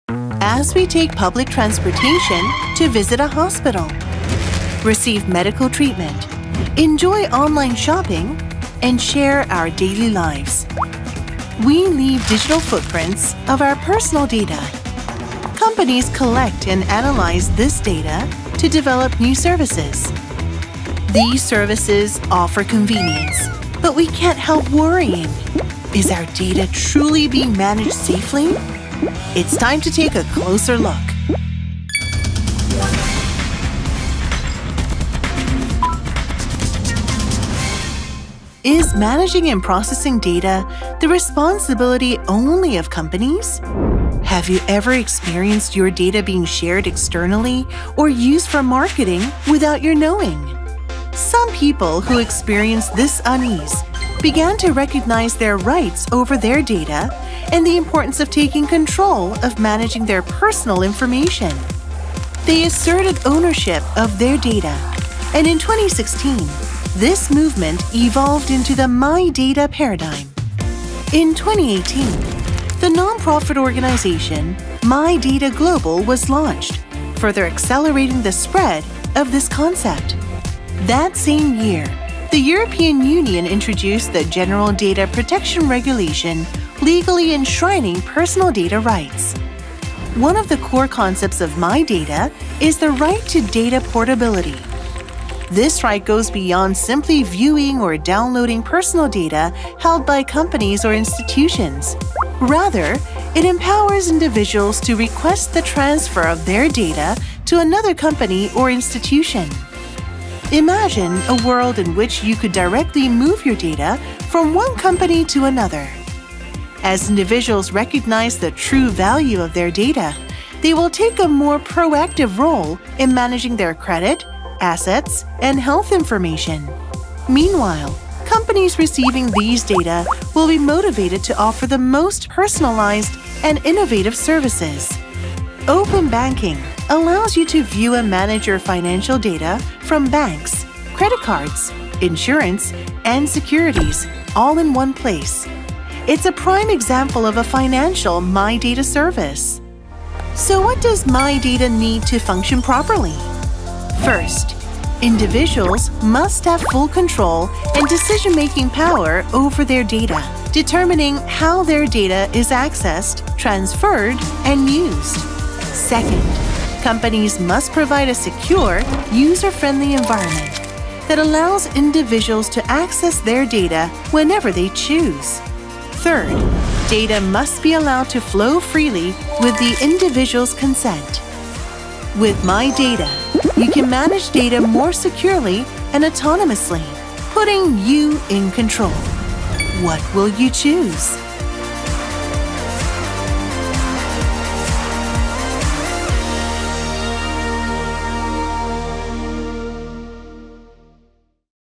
[KDI EIEC] e-Economic Information Review: MyData
With MyData, you can manage data more securely and autonomously, putting you in control.

00:00 Intro
00:39 Background of the emergence of MyData
01:30 Right to Data Portability
02:30 Necessary conditions for the activation of MyData